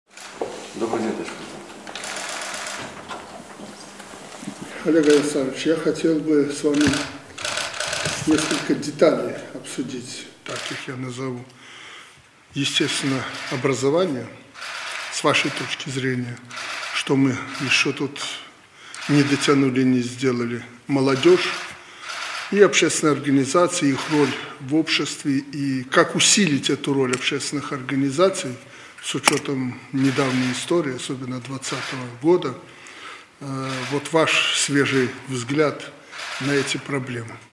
Лукашенко: Что мы ещё тут не дотянули, не сделали?
Лукашенко: С вашей точки зрения, что мы ещё тут не дотянули, не сделали? Молодёжь. И общественные организации, их роль в обществе. И как усилить эту роль общественных организаций с учётом недавней истории, особенно 2020 года. Вот ваш свежий взгляд на эти проблемы!

Вопросы сферы образования и работу общественных организаций обсудили 15 апреля на встрече у Президента Беларуси Александра Лукашенко. На встречу с главой государства были приглашены глава Администрации Президента Беларуси Игорь Сергеенко и ректор Полоцкого государственного университета, сенатор Олег Романов.